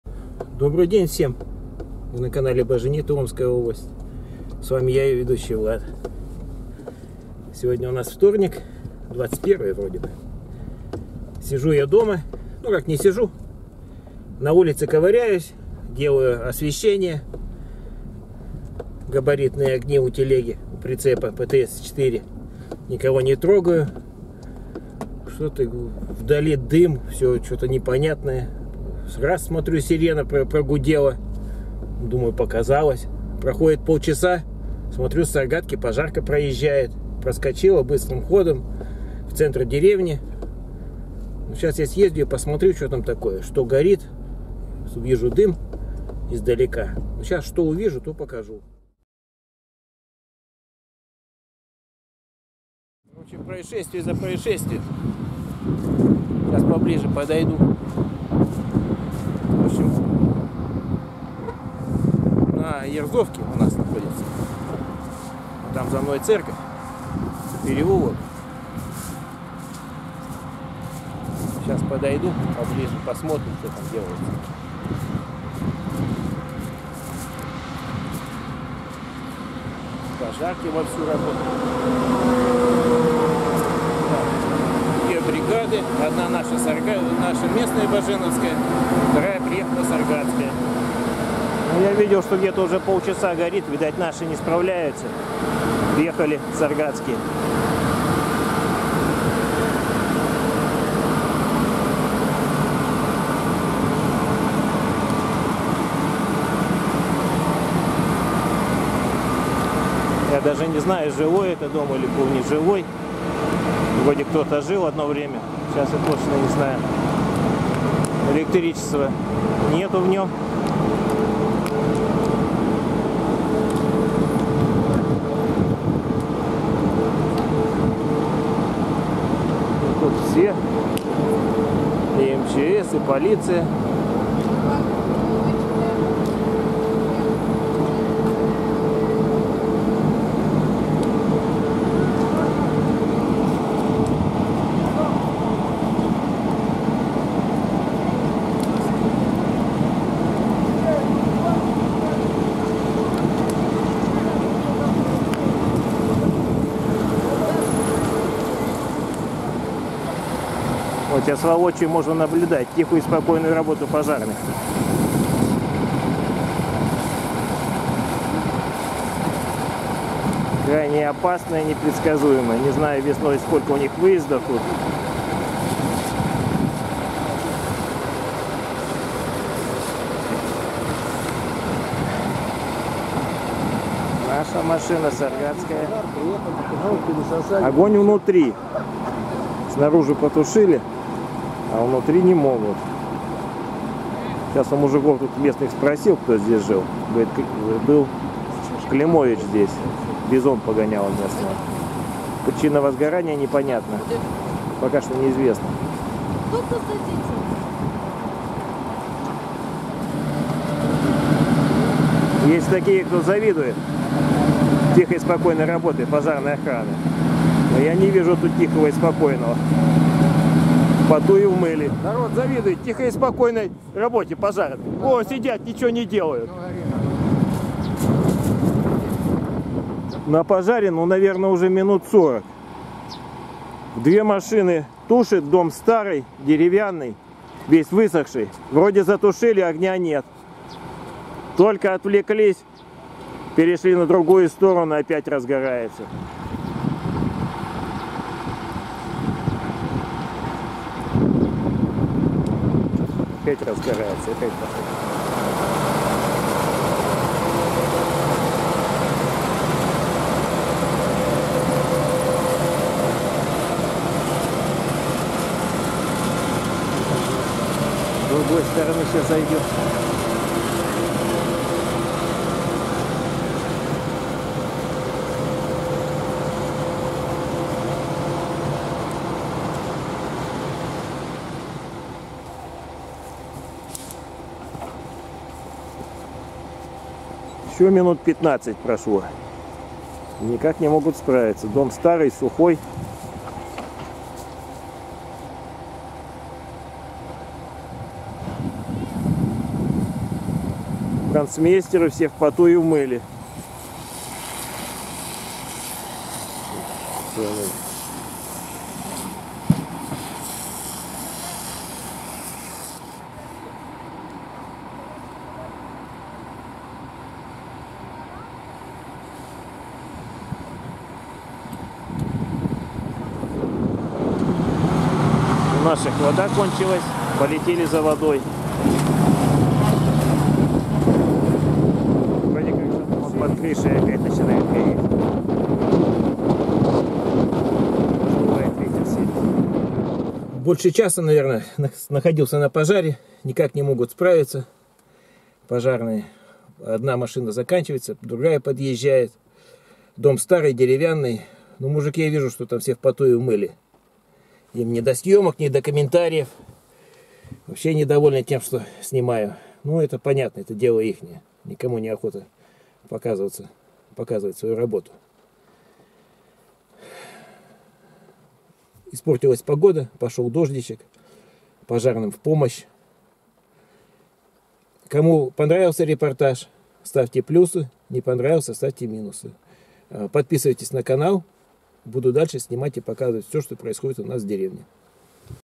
Пожарные тушат пожар Баженово Саргатского р Омская обл
Тушение дома на улице Советской(ерзовке) в селе Баженово Саргатского района Омская область.
Во время тушения было задействовано две  бригады пожарных. Баженовского противопожарного участка и Саргатской  МЧС. Пожарная Охрана 55 ПСЧ р.п. Саргатское.
Пожар был своевременно потушен, человеческих жертв нет.   Возгарание соседних домовладений допущено не было. популярныйц видео блогер Саргатского района Омской области ведущий канала Бажениты Влад, освещает, как  пожарные тушили возгарание частного домовладения в селе Баженово .